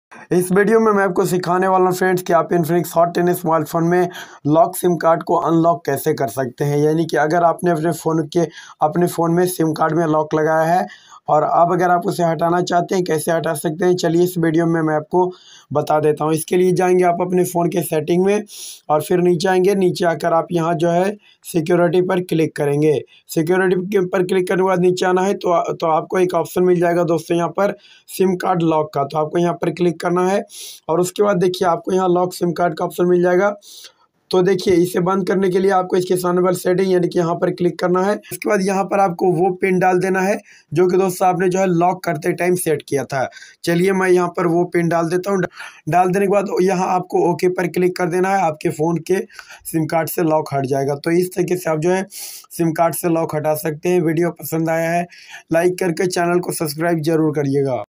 infinix hot 10s sim card se lock kaise hataye ।। how to unlock sim card in infinix Hot 10s
is video me bataya gaya hai friends ki Infinix Hot 10s mobile me sim card se lock Kaise hataya jata hai